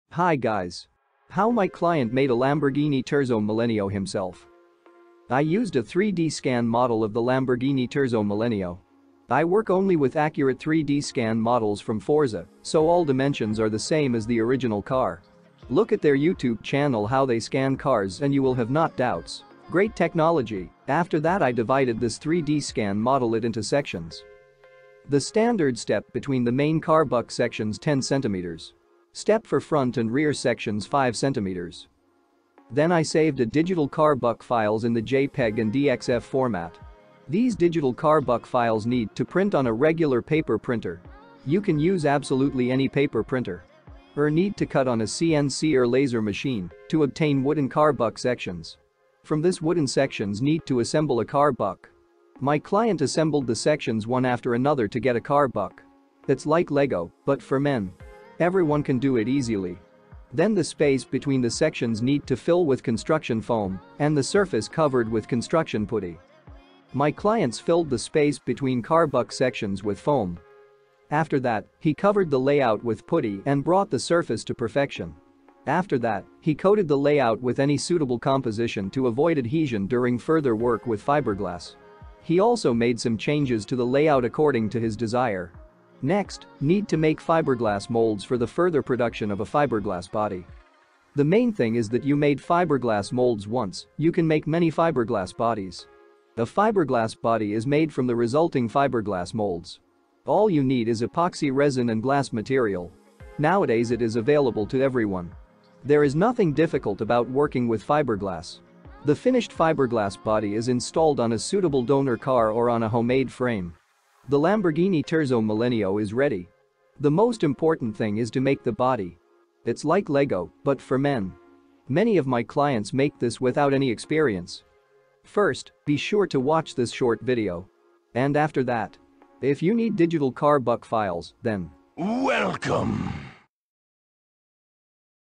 🔥🛠️DIY Lamborghini Terzo Millennio Kit Car With/Out CNC Machine & With/Out 3D printing & Restoration
Full 100 days build about how to make DIY Lamborghini Terzo Millennio supercar or kit car in real life from digital car buck files and how to make fiberglass body from epoxy resin. Woodworking projects with/out CNC machine and with/out 3D printer. Built not bought philosophy and restoration

- Our web store with digital car buck files for any car self-building